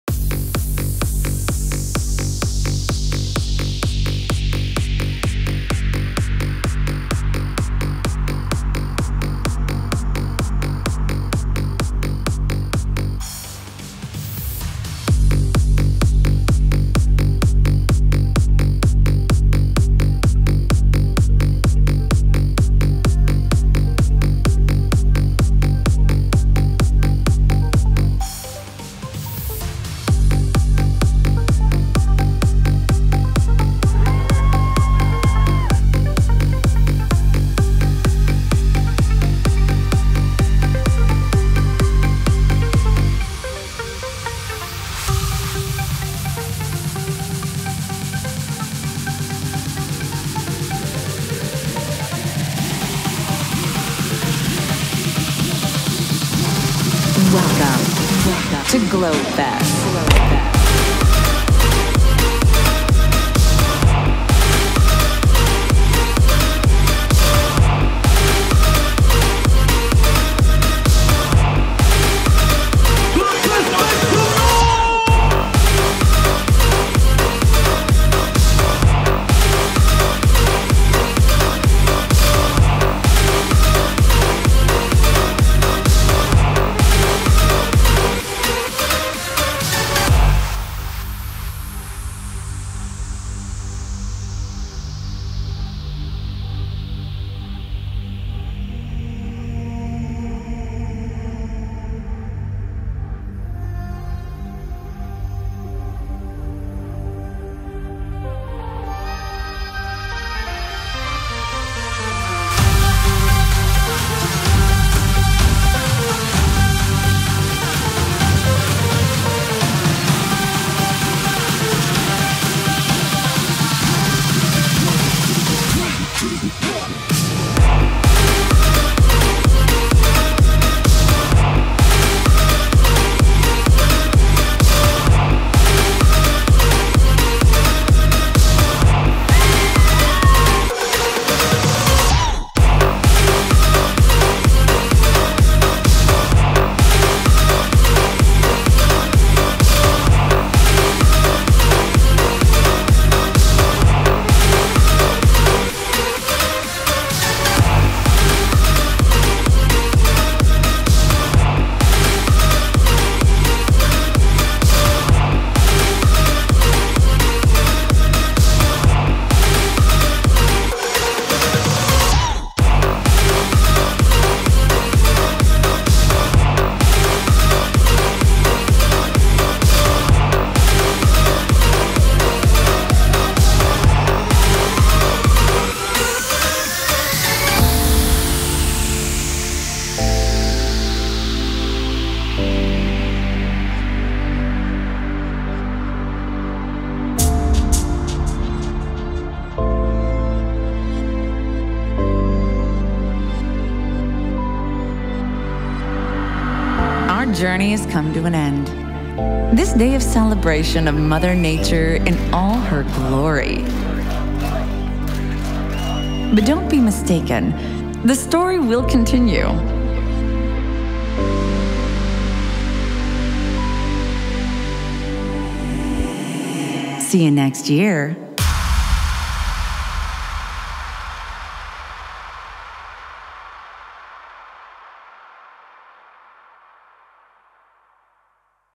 GlobeFest 2015 - The 4 Elements (Official Aftermovie)
Aftermovie I shot in 2015, for a dance fetival in my hometown.

Camera used: Canon EOS 550D (T2i)
Drone: DJI Phantom 2 (GoPro 3 black)

(Under my previous name "Red Eye Media")